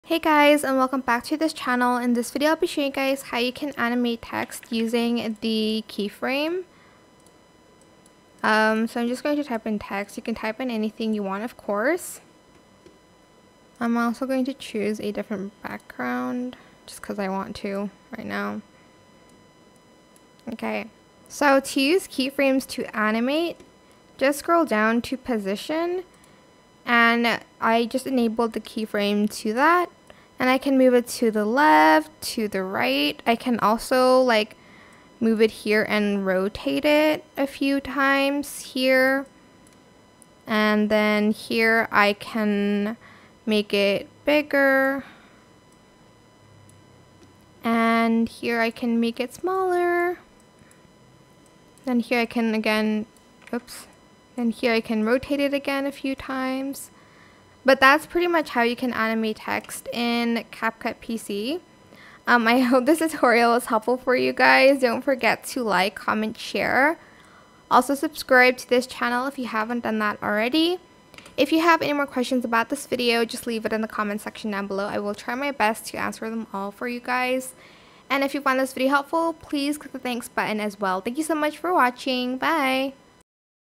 ❤ How to Animate Text with Keyframes in CapCut PC
Animate text with keyframes on CapCut PC by watching this tutorial.